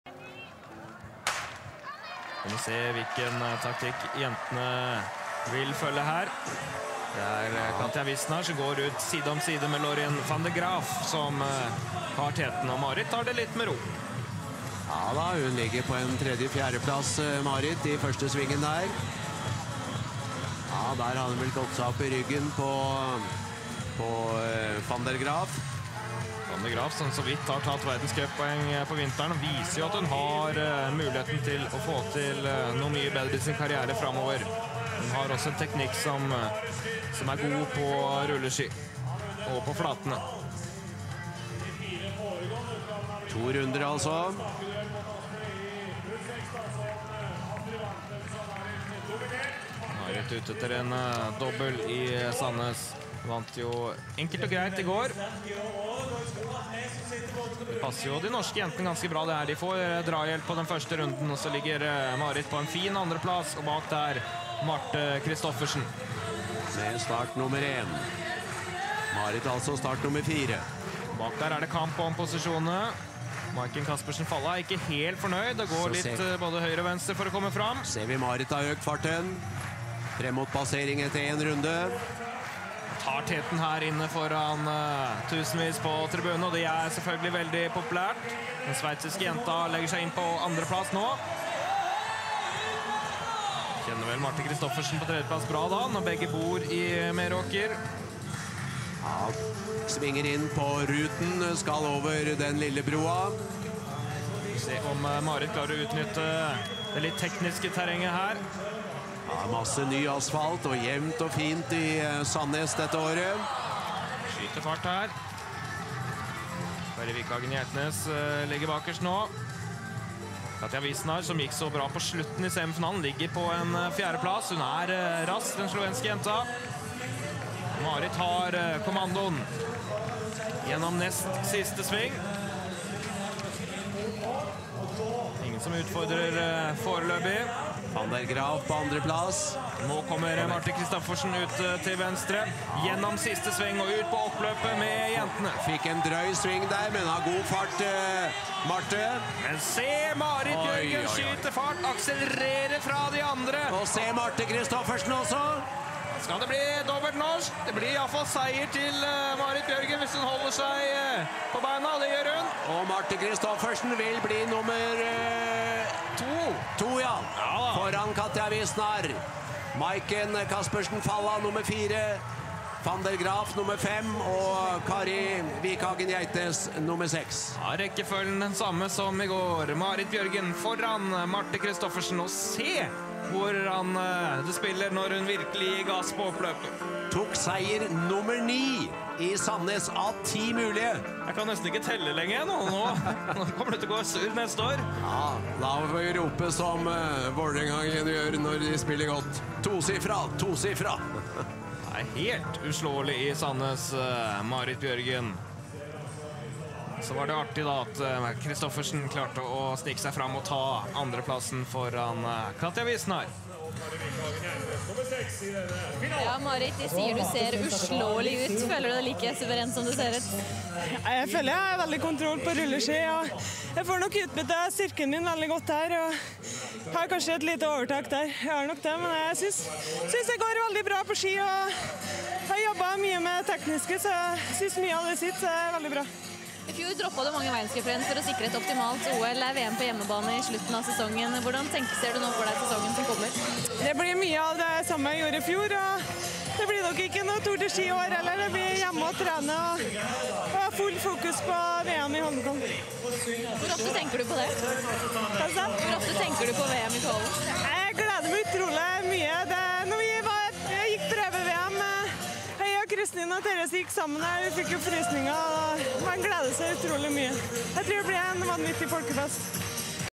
Blink 2010 Marit Bjørgen vinner sprint rulleski NRK1
Marit bjørgen vinner sprint på Blinkfestivalen i Sandnes 2010. 1. Marit Bjørgen 2. Marthe Kristoffersen 3. Katja Visnar 4. Maiken Caspersen Falla 5. Laurien Van Der Graaf 6. Kari Vikhagen Gjeitnes